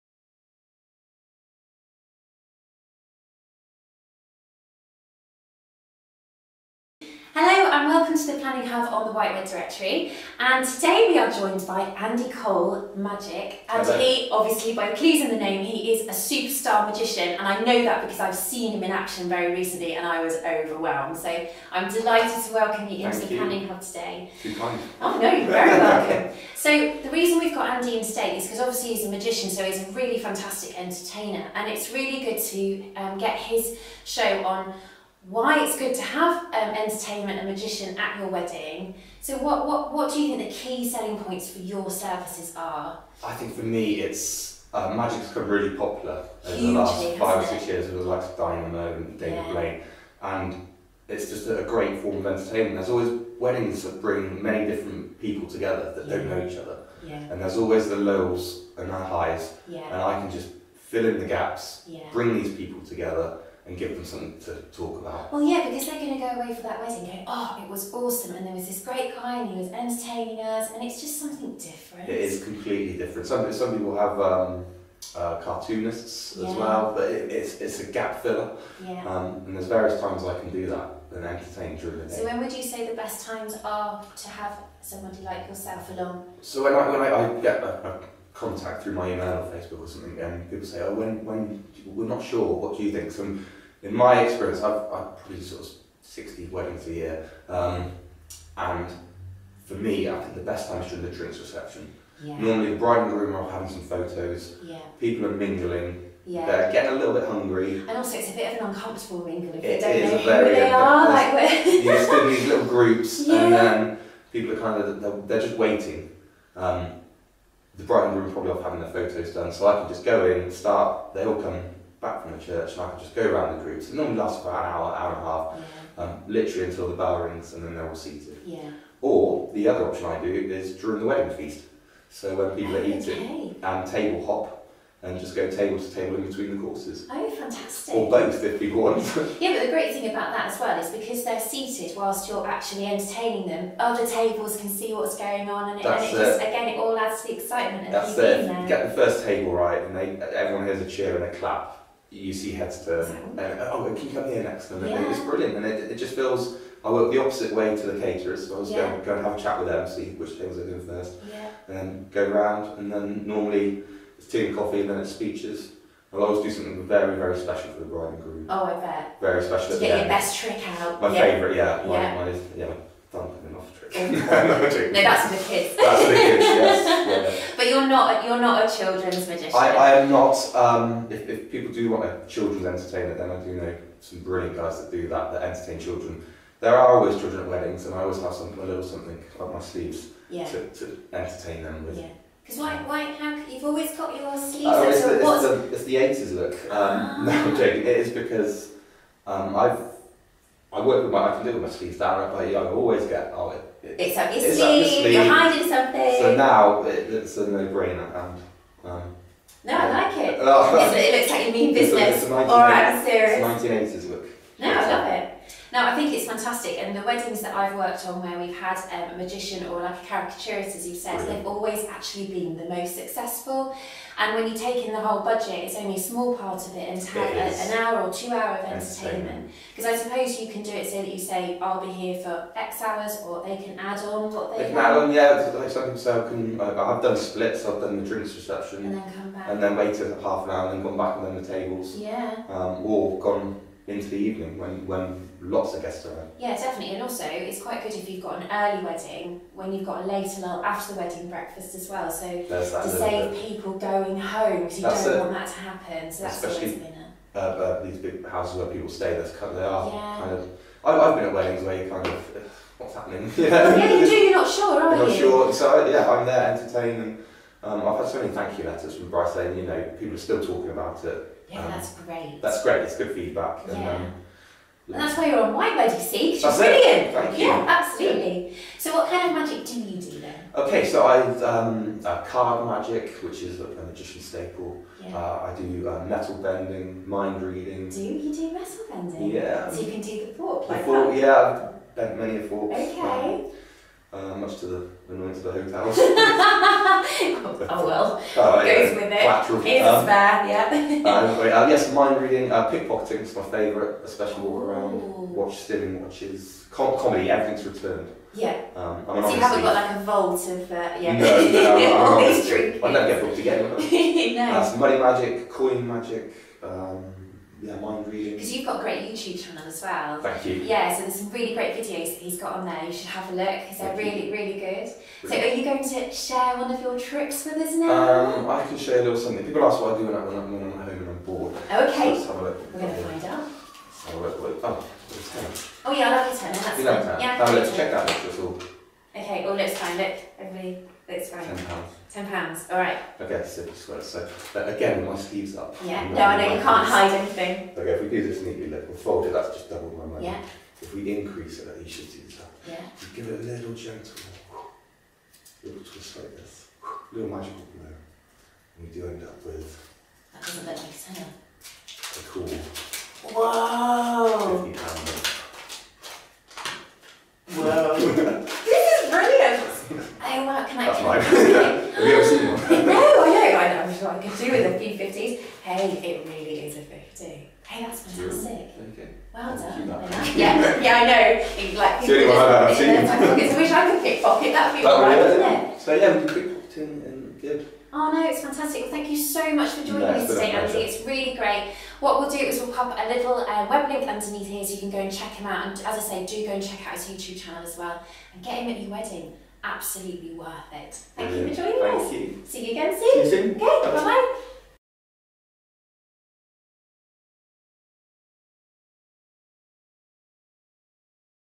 Andy Cole Magic joins us in The Planning Hub: Why choose magic entertainment for your wedding day
Whitewed Directory approved Andy Cole Magic gives his expert advice in The Planning Hub on the value of magic entertainment for your wedding day